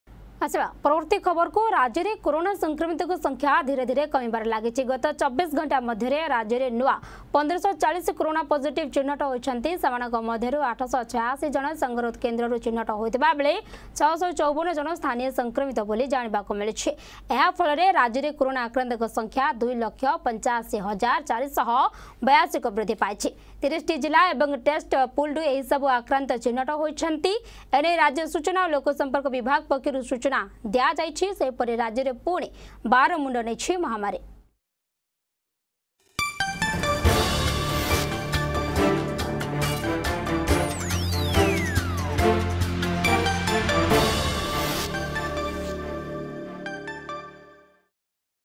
ରାଜ୍ୟରେ ଖସୁଛି କରୋନା ସଂକ୍ରମଣ।
ରାଜ୍ୟରେ କରୋନା ସଂକ୍ରମିତଙ୍କ ସଂଖ୍ୟା ଧୀରେ ଧୀରେ କମିବାରେ ଲାଗିଛି। ଗତ ୨୪ ଘଣ୍ଟା ମଧ୍ୟ‌ରେ ରାଜ୍ୟରେ ନୂଆ ୧୫୪୦  କରୋନା ପଜିଟିଭ ଚିହ୍ନଟ ହୋଇଛନ୍ତି।